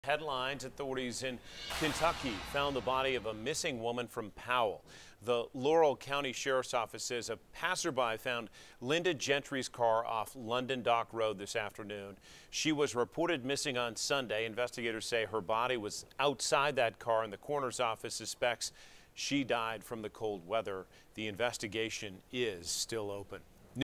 Body of missing Powell woman found in KY
Authorities in Kentucky found the body of a missing Powell woman.